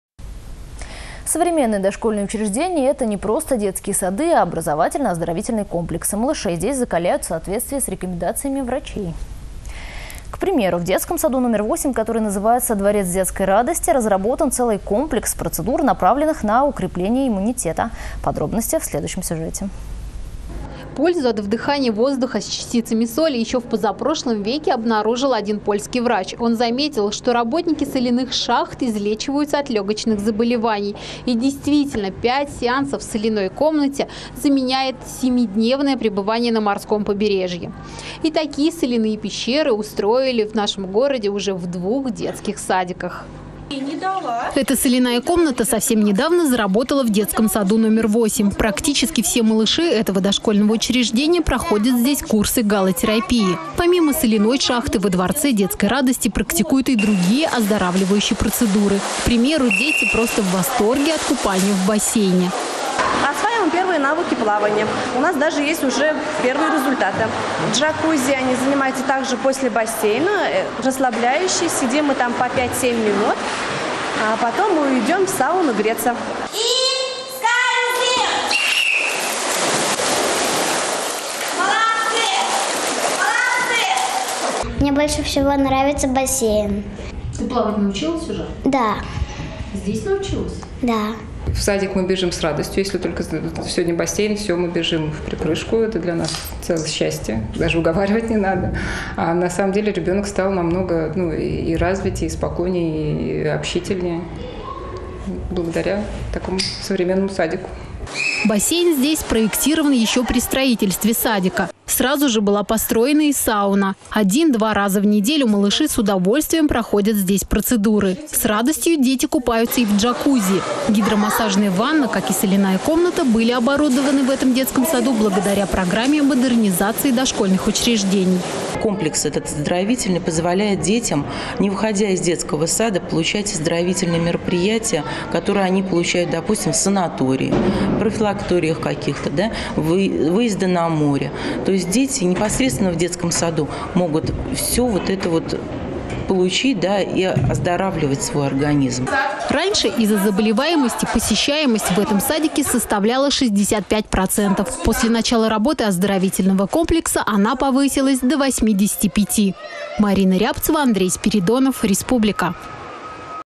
Галотерапия в детсаду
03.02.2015 Современные дошкольные учреждения — это не просто детские сады, а образовательно-оздоровительные комплексы. Малышей здесь закаляют в соответствии с рекомендациями врачей. К примеру, в детском саду № 8, который называется  «Дворец детской радости», разработан целый комплекс процедур, направленных на укрепление иммунитета.